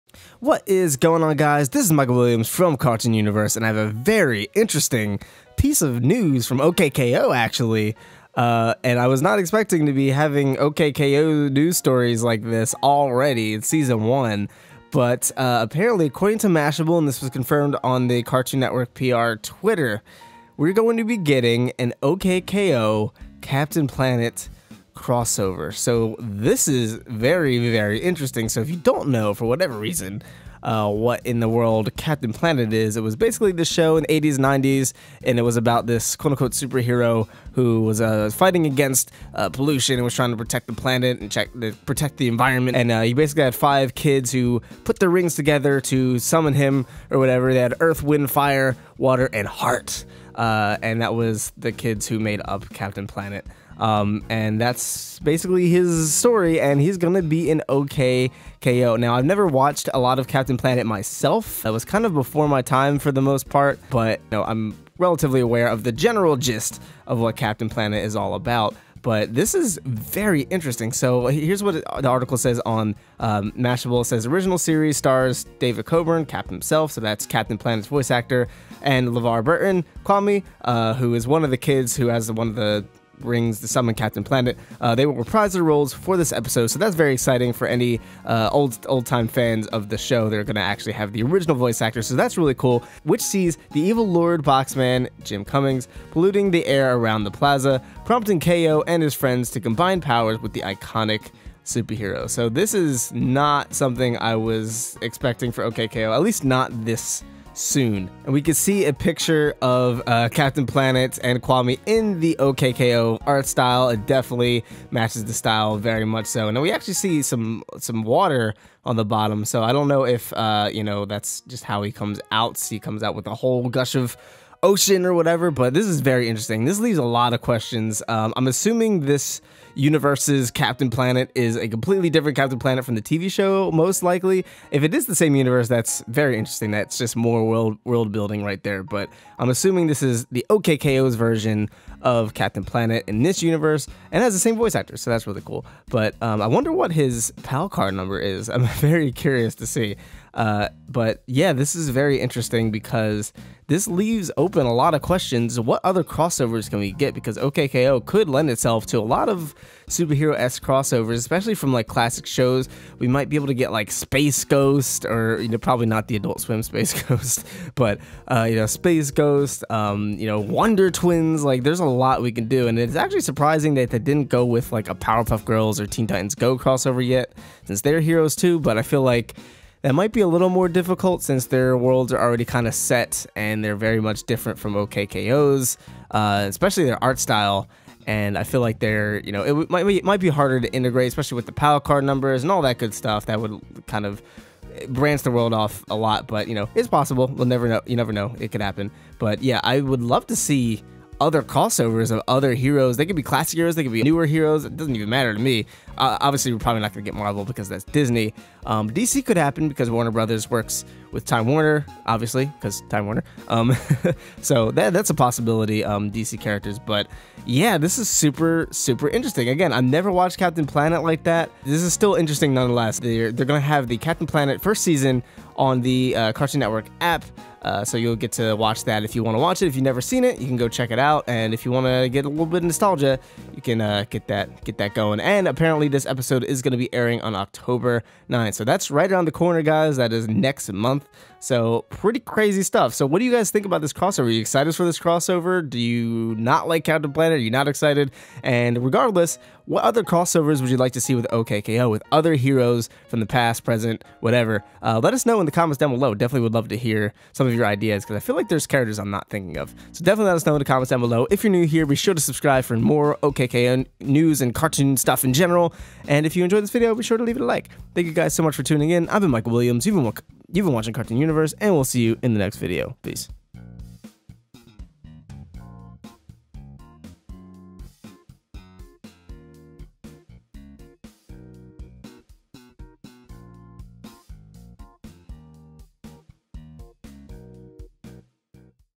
OK KO! x Captain Planet CROSSOVER!? - OK KO! Let's Be Heroes News
So it looks like KO and the rest of the Lakewood Plaza Turbo gang are going to be teaming up with a classic hero, Captain Planet (and Kwame) to defeat Boxman! Let's talk about the details.